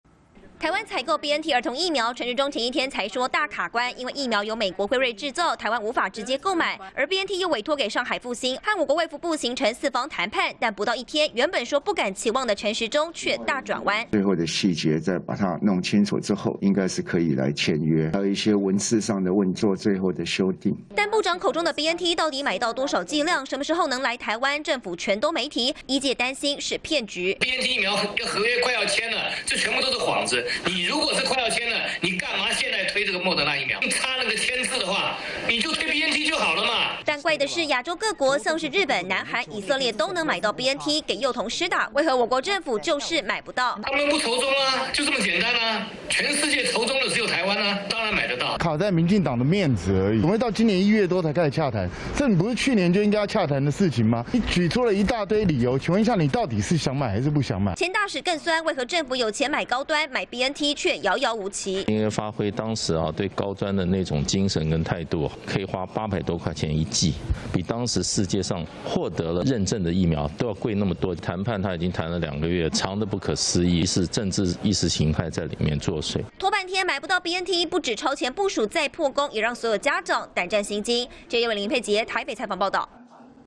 買"BNT兒童疫苗"有譜?! 指揮中心:很快就能簽約│中視新聞 20220420
為何世界各國幾乎都買到兒童BNT，但台灣卻卡關，指揮中心陳時中透露，現在簽約有譜了，台灣也能買到兒童BNT，不過能買到多少量、何時來通通不知道，醫界擔心恐怕都是幌子。